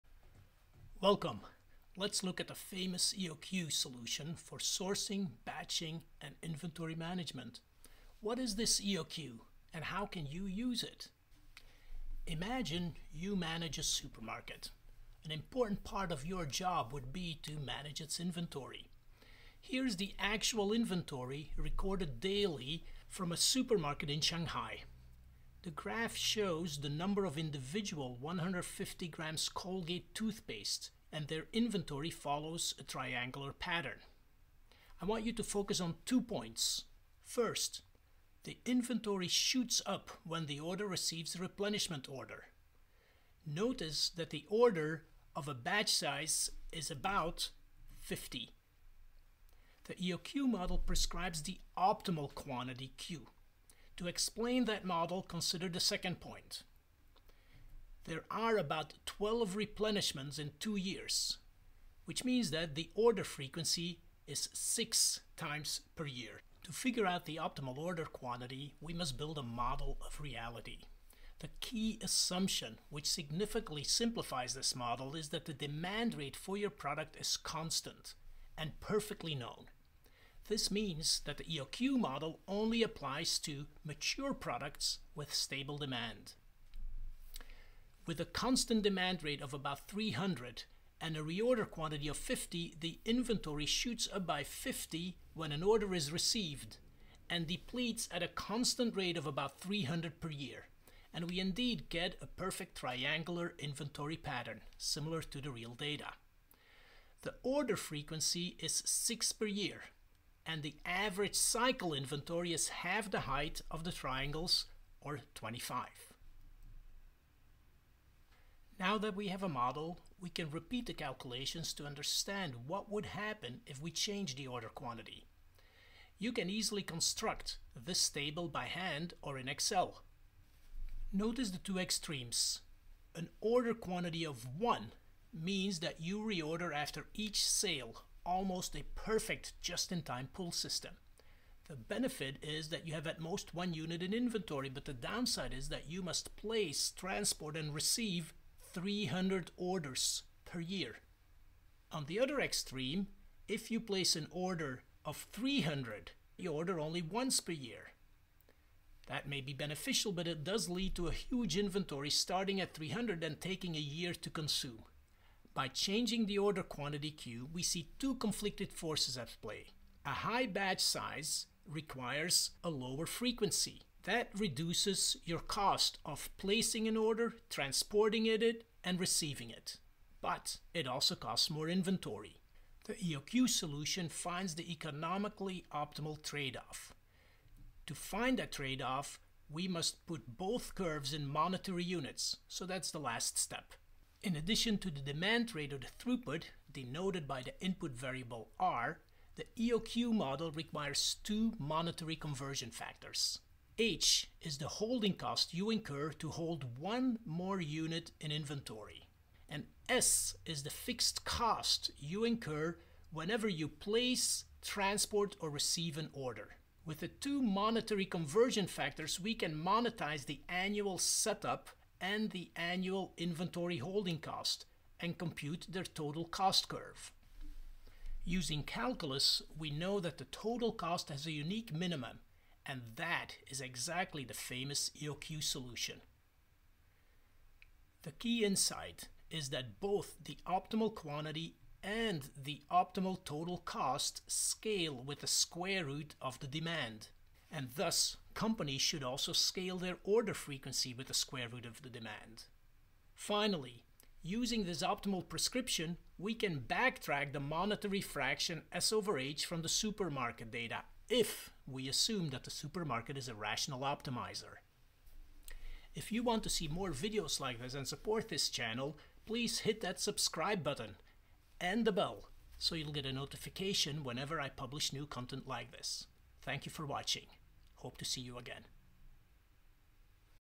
What's that "EOQ solution" for sourcing, batching and inventory management? (4min)
This video considers two key decisions in inventory management: how much and when to order?  Starting from real data, we build a model to optimize the total cost, which is the sum of the setup (order + transport + receiving) cost and holding cost.  The solution is the celebrated Economic Order Quantity.